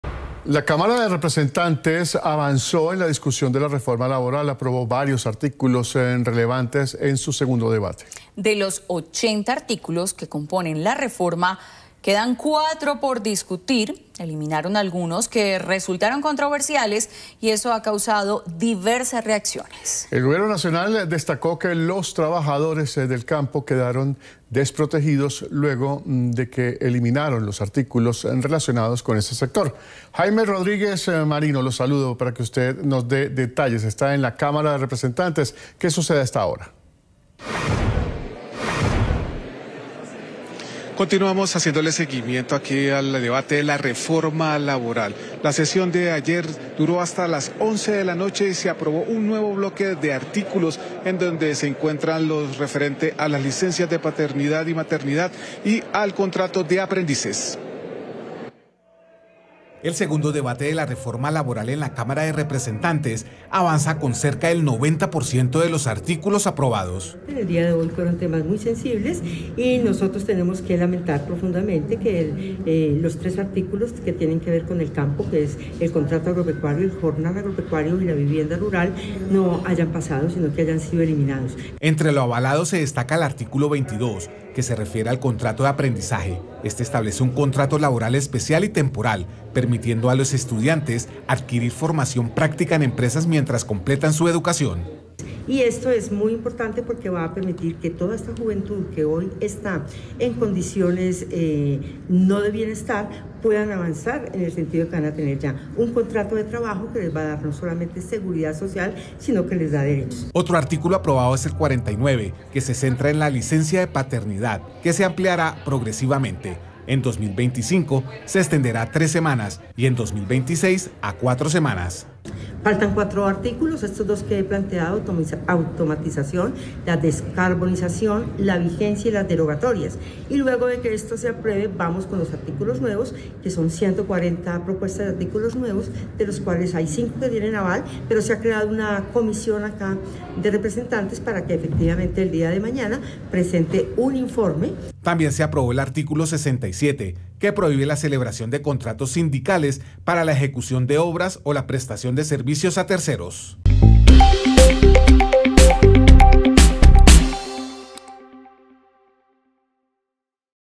Cámara aprueba cerca del 90 % de los artículos de la reforma laboral
La reforma laboral del gobierno de GustavoPetro avanza en la Cámara de Representantes con cerca del 90 % de los artículos aprobados.